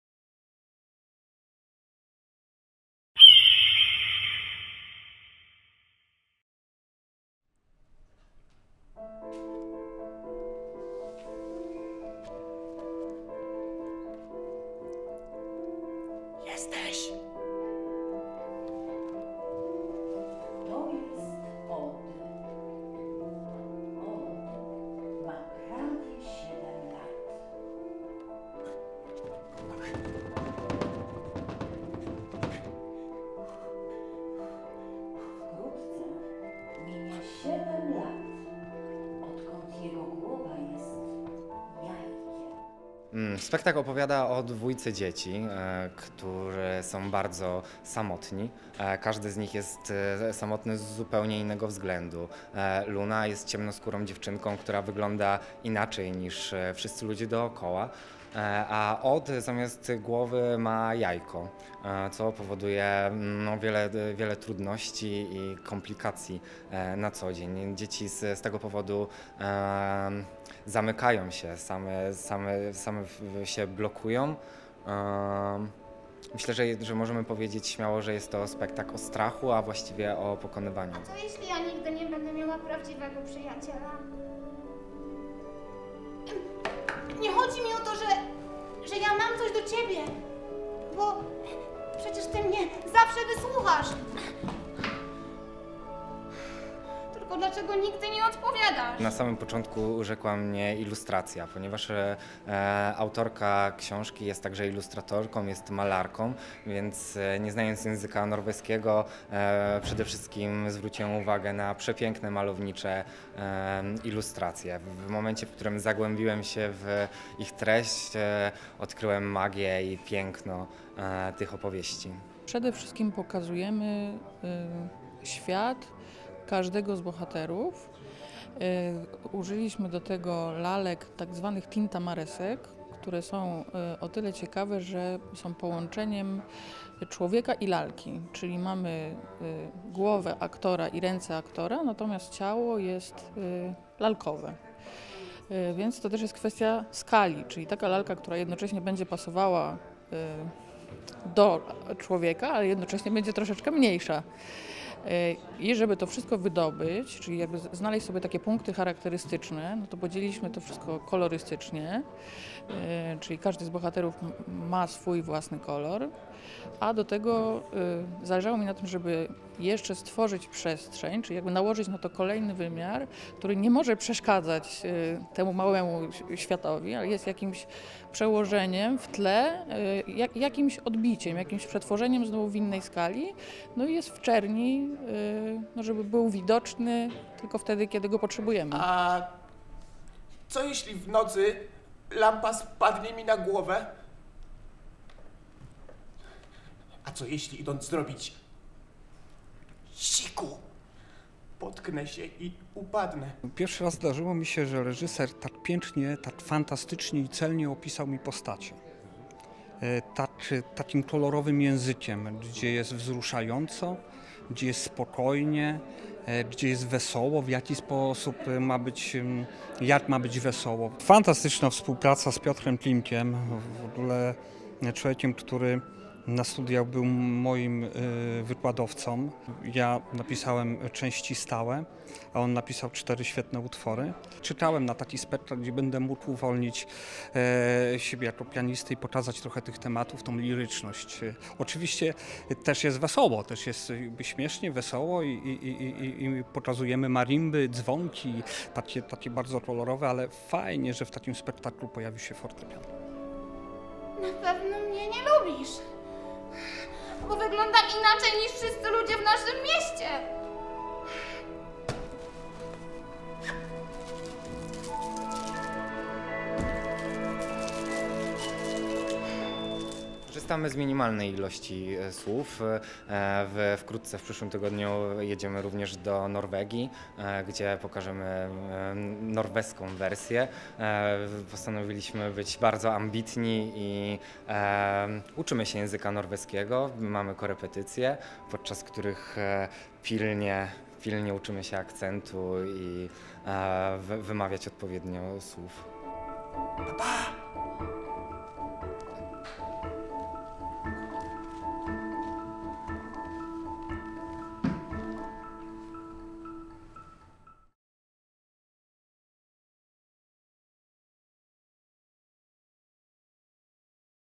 Odd i Luna - Wrocławski Teatr Lalek
pektakl na podstawie książek Lisy Aisato ODD ER ET EGG i EN FISK TIL LUNA.

Odd ma prawie siedem lat. Siedem lat, odkąd jego głowa jest... jajkiem. Odd bardzo się o nią boi. Bo co, jeśli spadnie na niego lampa? Albo potknie się, idąc w nocy do toalety? Jego życie jest trudne – wciąż musi na siebie uważać. Pewnego dnia spotyka w lesie dziwną dziewczynkę w przebraniu pszczoły. Od tej chwili jego los zupełnie się odmieni.

Ciemnoskóra Luna jest bardzo nieśmiała. Czuje, że wygląda inaczej niż ludzie dookoła. Boi się, że dzieci będą wytykać ją palcami i dlatego rzadko wychodzi z domu. Pewnej nocy dostrzega za oknem latającą rybę. Dziewczynka domyśla się, że tajemniczy gość zgubił drogę do swojego domu. Chciałaby pomóc rybie, ale jak to zrobić, gdy ta wypowiada tylko pojedyncze, niezrozumiałe słowa?

Odd i Luna czują się samotni i nieakceptowani przez otoczenie. Codziennie rozmawiają z księżycem i czekają, aż w ich życiu coś się zmieni. Dopiero spotkanie z Innym pozwoli im uwierzyć w siebie i otworzyć na świat. I nie będzie to kwestia magii czy czarów, a jedynie – serca.

Przemysław Jaszczak, młody reżyser z bogatym teatralnym doświadczeniem, postanowił połączyć w jednym przedstawieniu dwie norweskie opowieści Lisy Aisato, nie wiążąc ich jednak fabularnie. Powstał baśniowy spektakl z minimalną liczbą słów, oparty na obrazie, opowiadający o inności i poszukiwaniu przyjaźni oraz własnego miejsca w świecie.

Adaptacja, reżyseria: Przemysław Jaszczak
Scenografia, lalki, kostiumy: Zuzanna Srebrna
Projekcje: Agnieszka Waszczeniuk
Reżyseria światła: Maciej Iwańczyk
Muzyka: Tomasz Lewandowski
Piosenki: Konrad Dworakowski (tekst), Piotr Klimek (muzyka)

Obsada: Grzegorz Borowski, Aneta Głuch-Klucznik, Agata Kucińska, Anna Makowska-Kowalczyk, Michał Szostak (gościnnie)